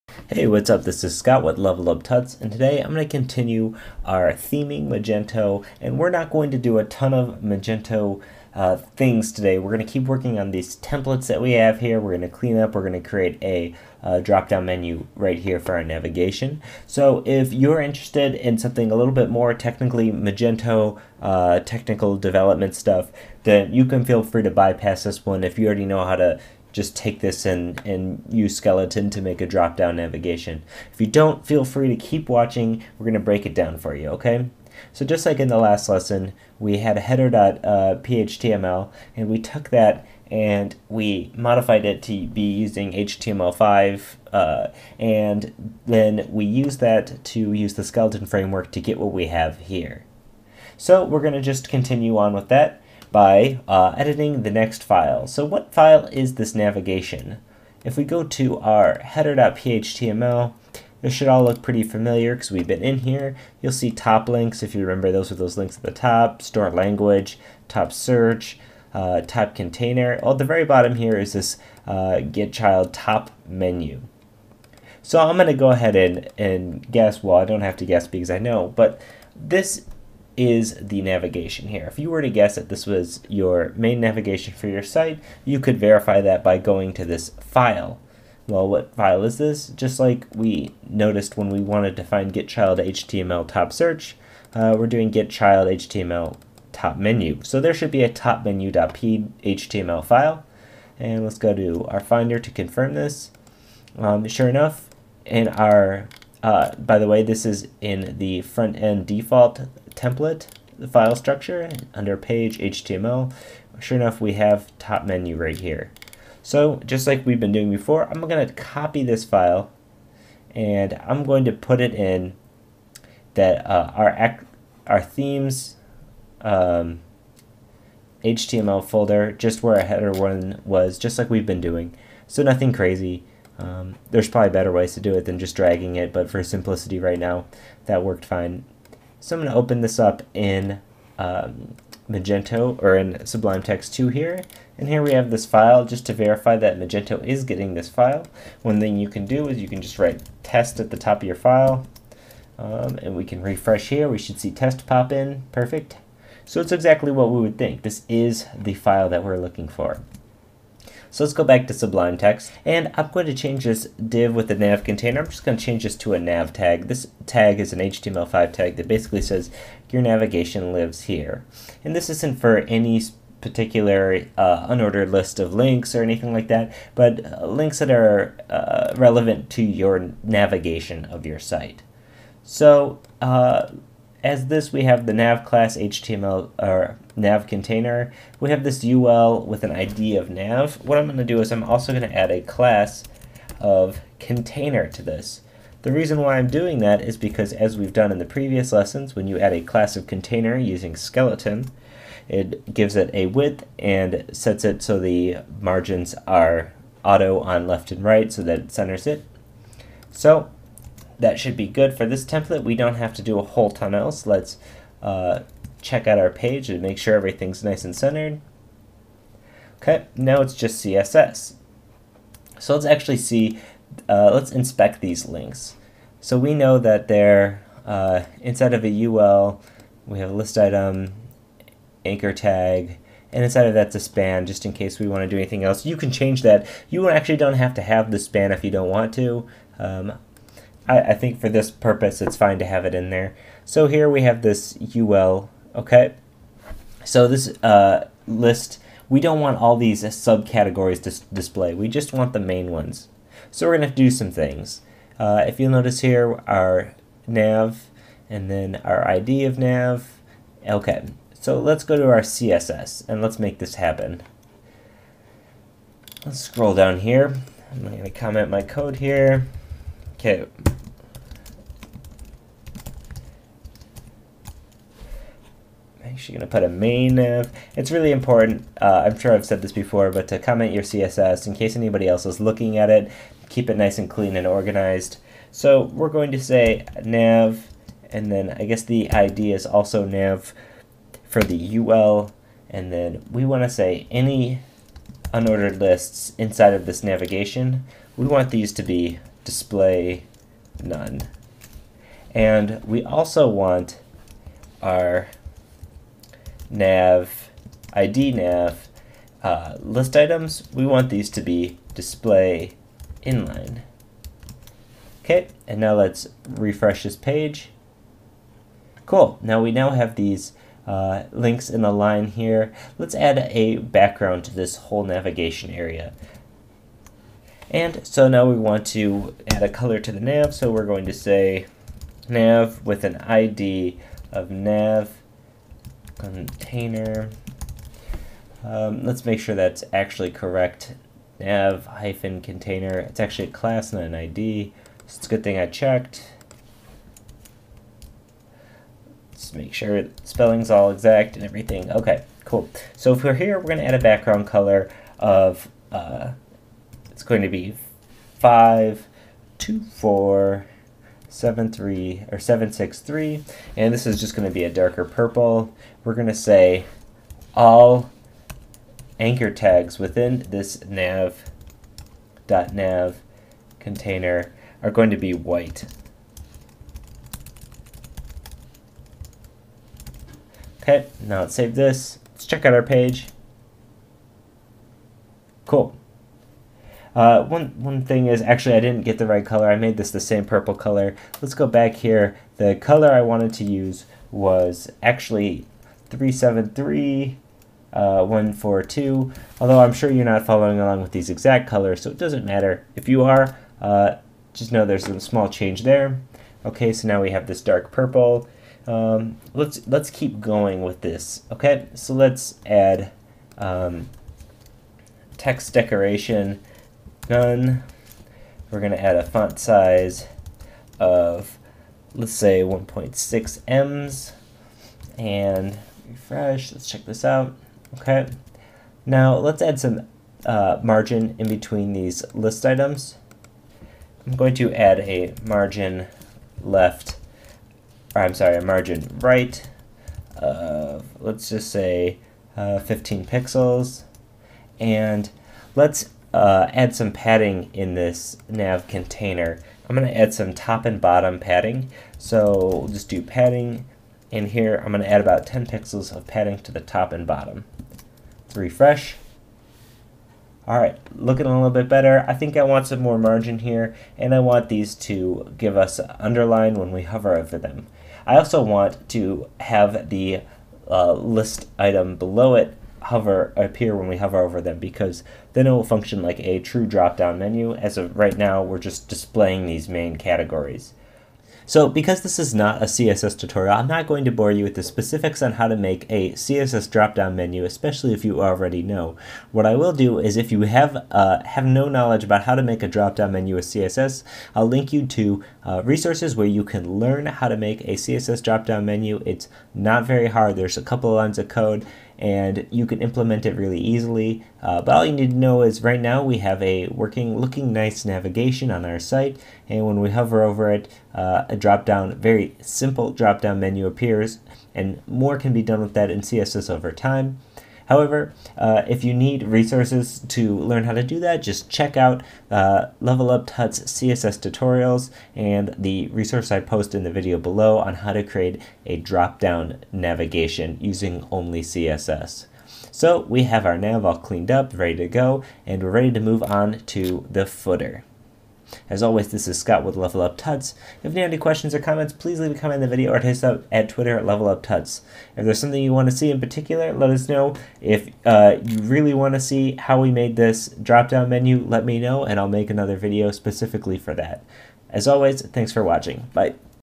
Theming Magento Community #15 - Cleaning Up Our Navigation
The 15th tutorial on Theming Magento. This tutorial gives a basic intro to the structure of your Magento site so that we can begin theming.

The flexible, open source commerce platform that powers over 250,000 online stores worldwide